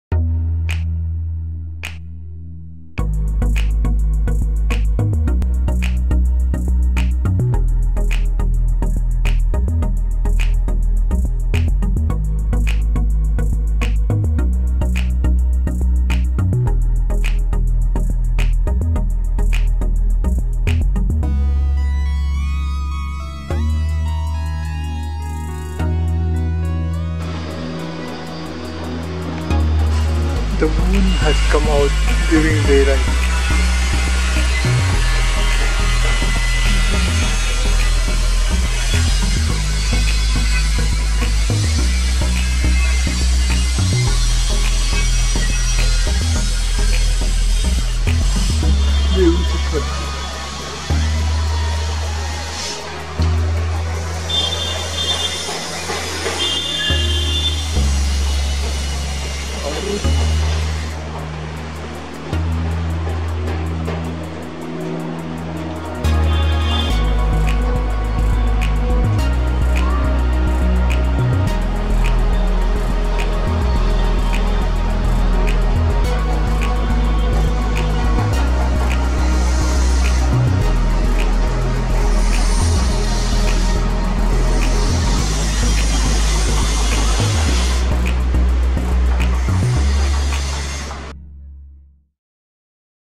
Waxing Gibbous Moon in Mizoram at Daylight by Lal Dingliana
Waxing Gibbous Moon in Mizoram at Daylight   on 13/12/2021
 zoomed by Lal Dingliana (Dingte)